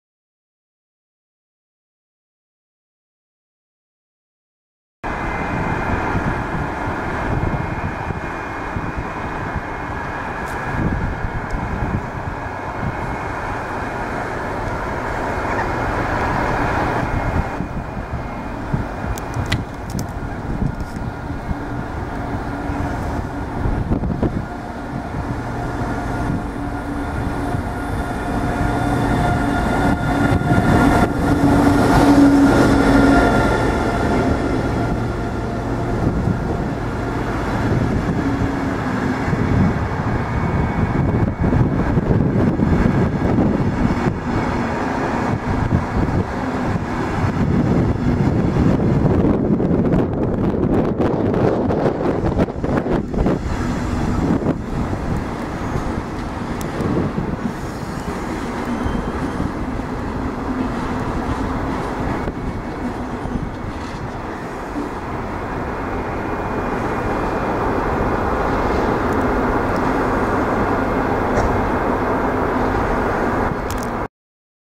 245 005 Munchen Hbf
DB 245 005 departing Munchen Hbf empty stock 18/6/2015